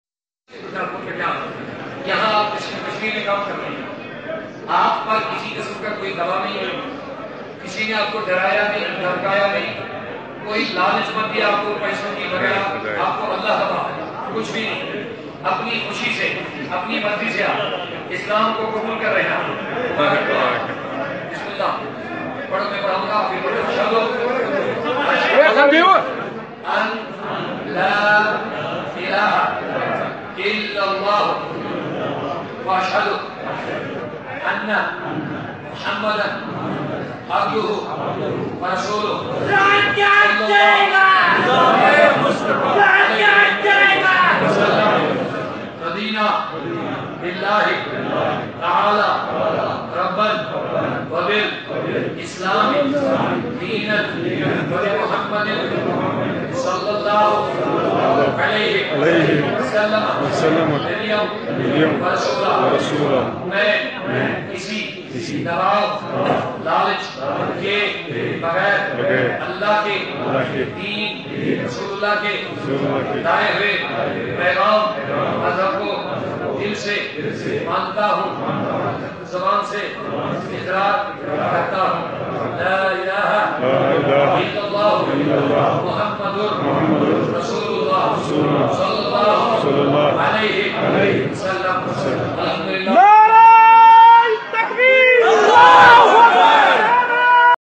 Dawoodi sab kay hatoon aj ek gair muslim bai ne kalima pada kabamarg islamabad main
please dont forget to subsribe
click on bell icon

25/05/2018
09 ramdhan Mubarak